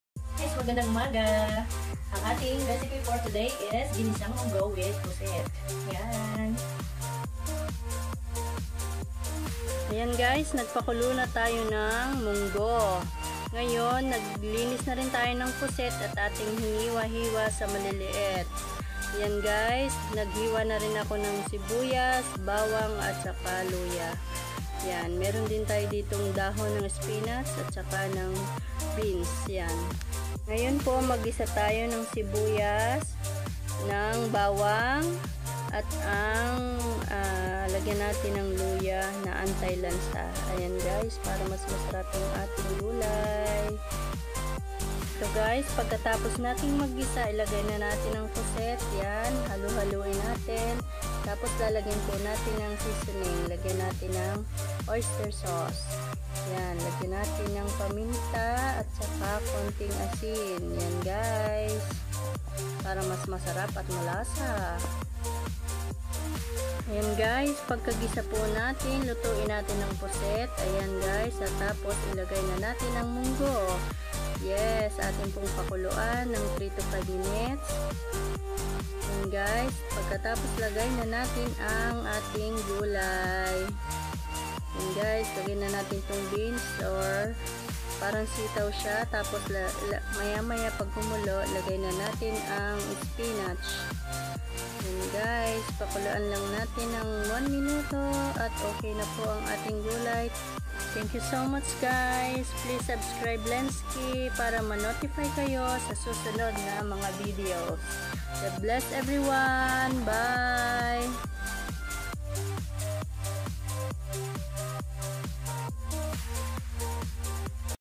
MONGGO WITH SQUIDS
Sharing my own recipe, you can watch and try to make a different taste.. super yummy and delicious..